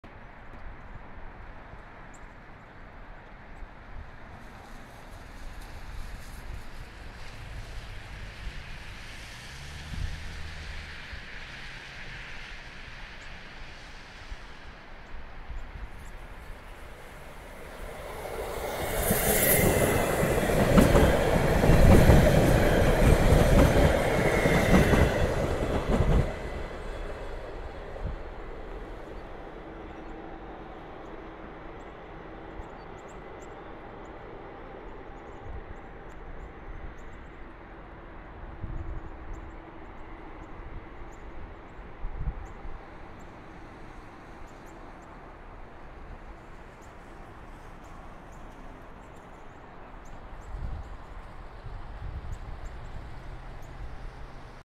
O-Train Line 2 Stadler FLIRT C16 passes over the VIA track towards Carleton
Yes ew digital zoom, had to fix a skill issue - I stumbled in the snow trying to keep the camera oriented correctly.